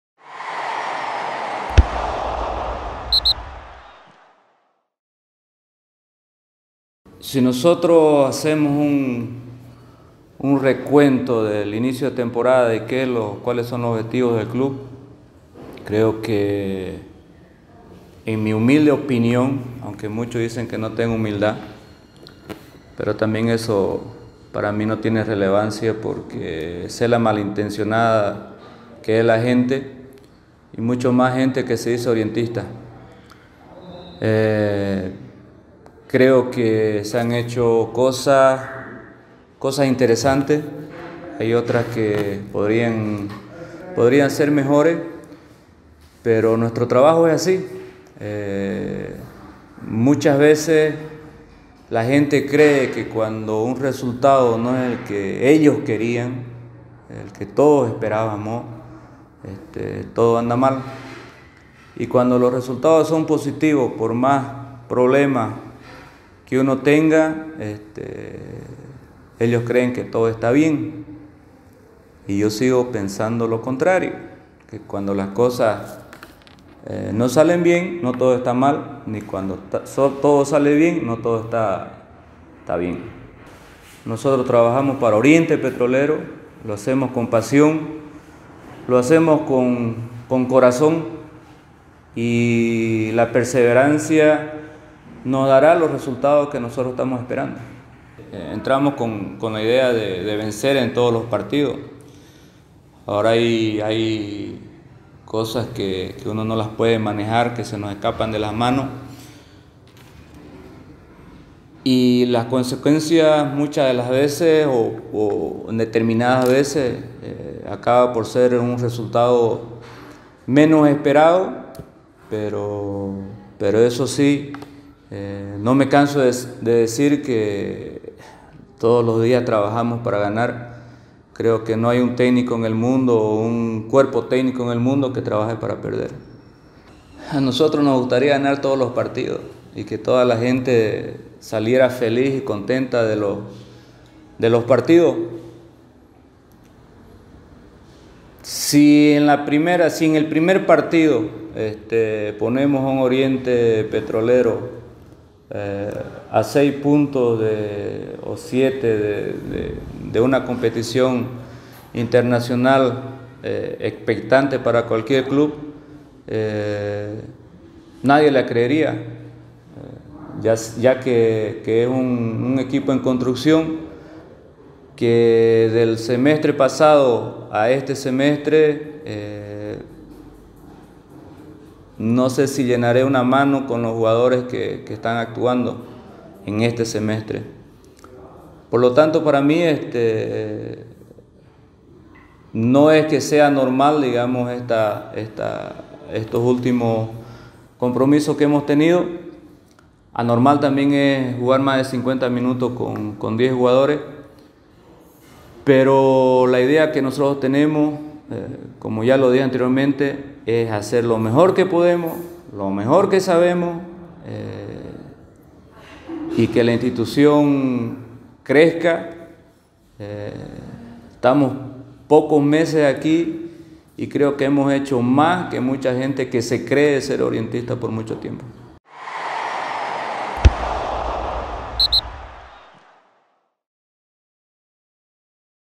Conferencia Erwin Sánchez
Erwin Sánchez hace un análisis de la situación de Club Deportivo Oriente Petrolero que perdió anoche 2 a 1 con CLUB INDEPENDIENTE PETROLERO